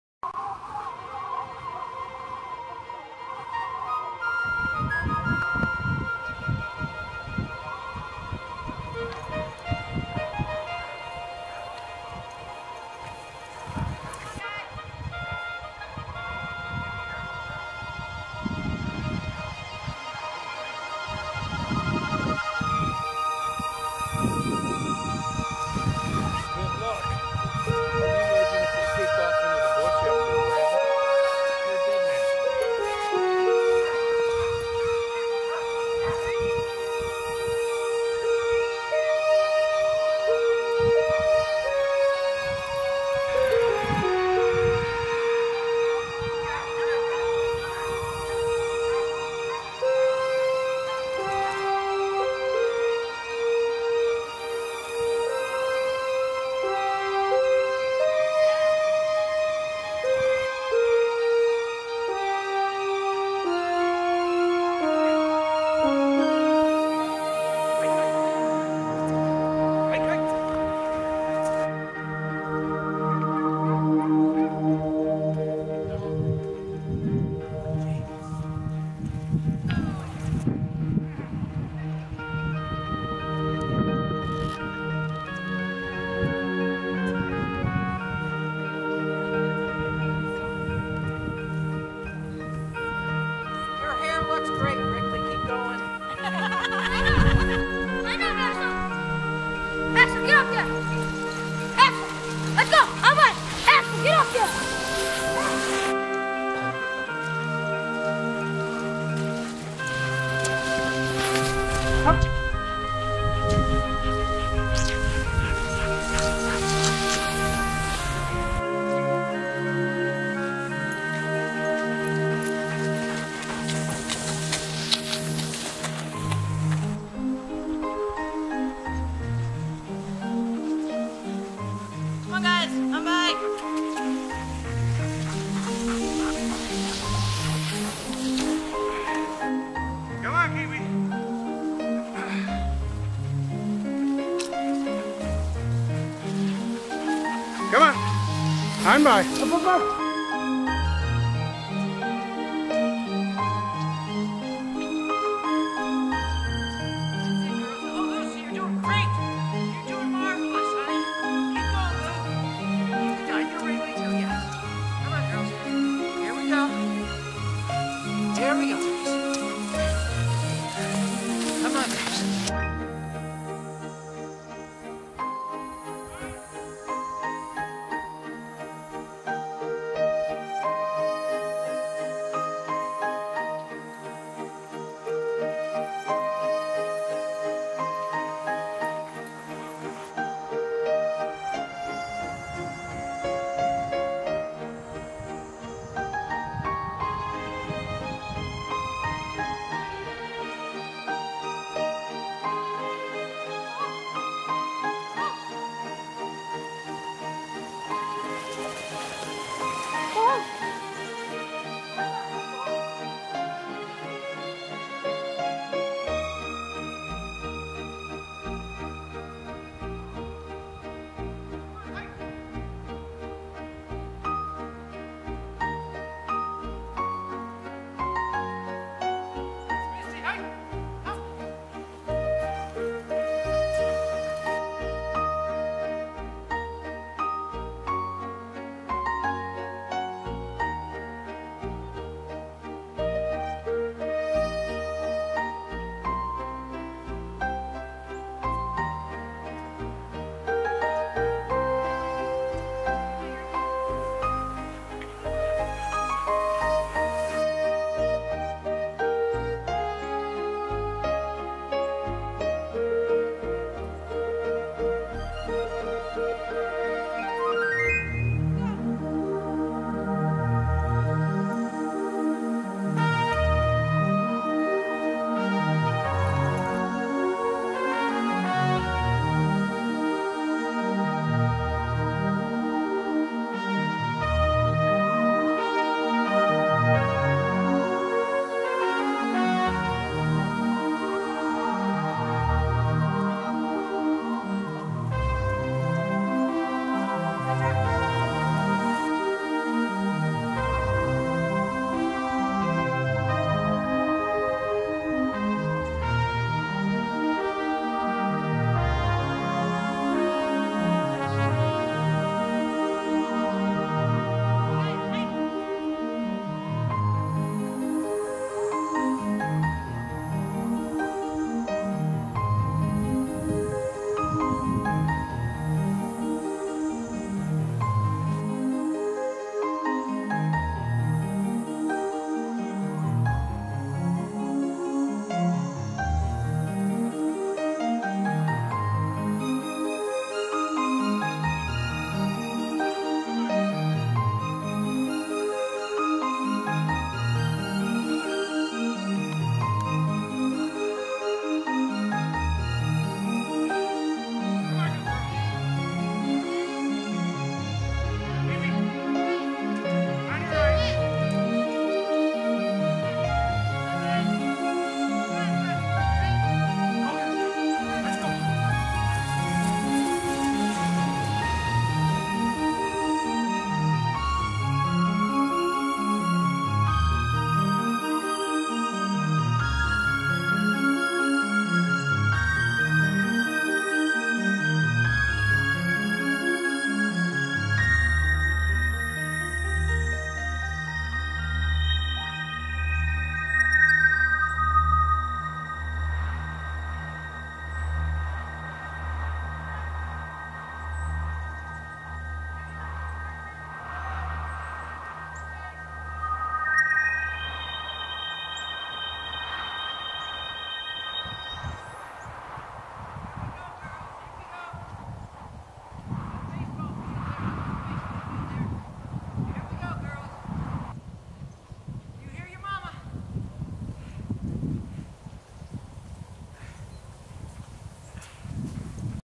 2011 - 3 Bear Sled Dog Race Skijoring
2011 - 3 Bear sled dog race held in Land O Lakes, Wisconsin.